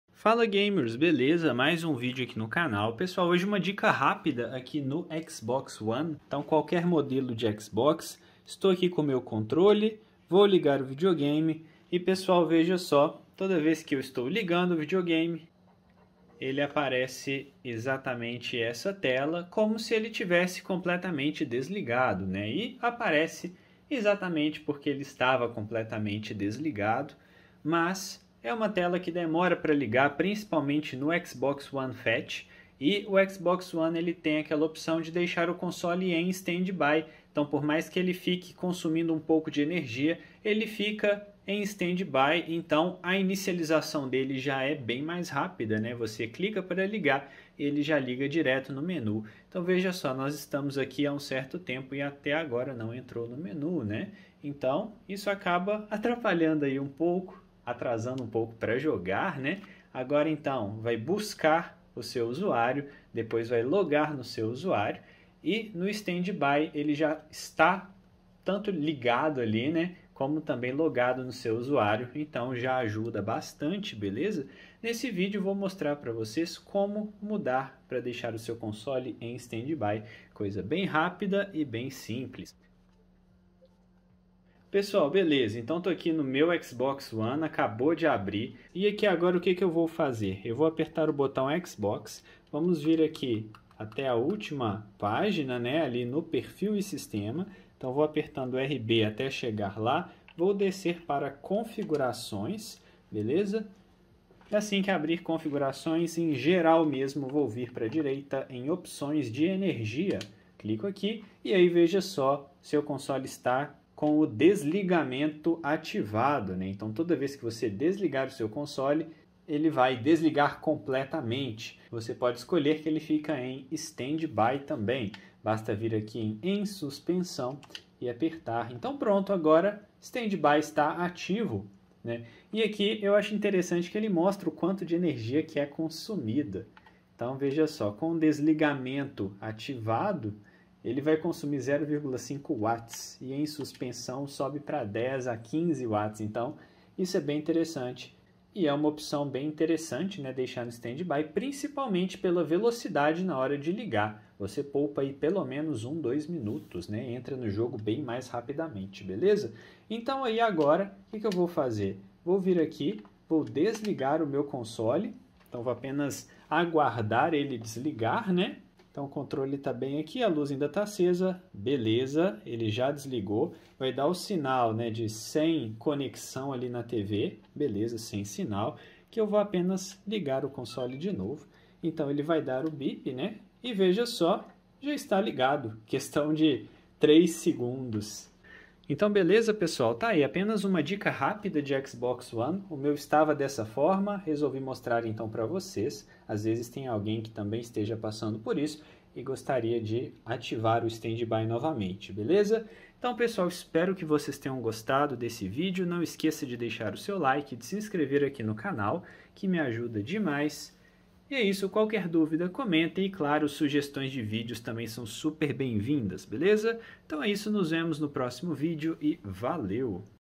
XBOX ONE - COMO ATIVAR O STANDBY - FAZER O CONSOLE LIGAR MAIS RÁPIDO
Fala gamers, beleza?

Hoje mostro como ativar o desligamento do console em standby!
Aumentando muito a velocidade para ligar depois.